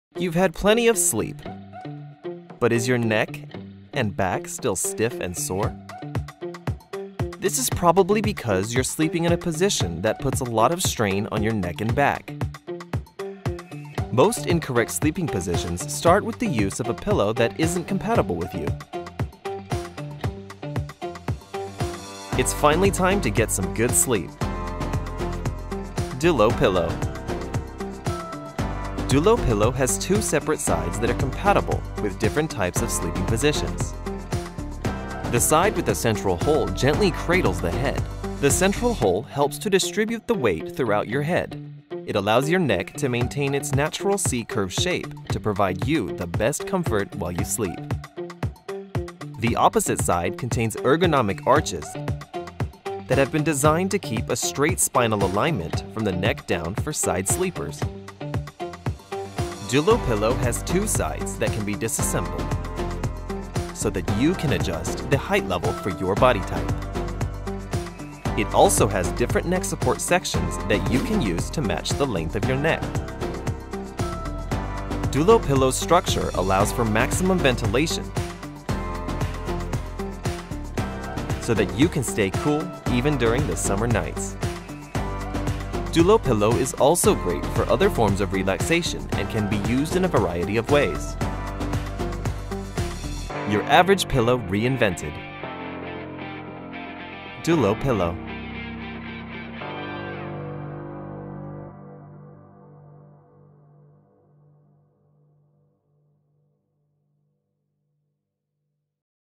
DULLO PILLOW: Your Average Pillow, Reinvented Dullo: Neck Relief Pillow for Back, Side | Indiegogo
DULLO PILLOW: Your Average Pillow, Reinvented

Dullo: Neck Relief Pillow for Back, Side | Indiegogo

Dullo: Neck Relief Pillow for Back, Side
Deep sleep made possible for all by our pillow's crater hole, ergonomic arches, neck support tubes.

With the built-in grooves and head-cradling hole in its ergonomic design, Dullo Pillow will ensure that you get a good night's sleep.